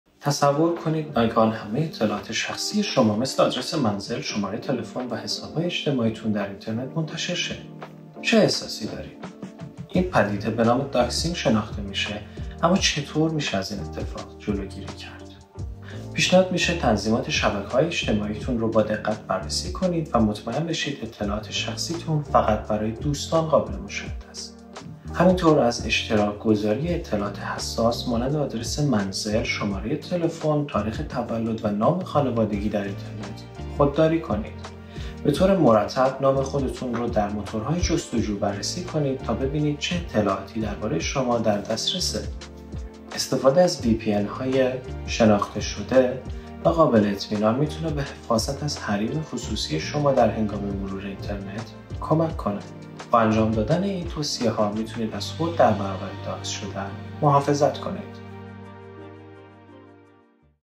یا می‌دانید داکسینگ چیست و چه خطراتی دارد؟
داکسینگ یا افشای اطلاعات شخصی، می‌تواند عواقب جدی برای حریم خصوصی شما داشته باشد. در ویدیوی امروز، با بررسی مفهوم داکسینگ و راهکارهایی برای جلوگیری از آن آشنا خواهید شد. با شناخت بهتر داکسینگ، می‌توانید از اطلاعات خود محافظت کنید و از عواقب ناگوار آن جلوگیری نمایید. در دنیای دیجیتال امروز، آگاهی از خطرات و حفظ حریم خصوصی امری ضروری است.

توصیه می‌کنیم برای افزایش آگاهی خود هایلایت «امنیت دیجیتال» را نیز در صفحه «فعالیت امن» مرور کرده و با دیگران نیز به اشتراک بگذارید.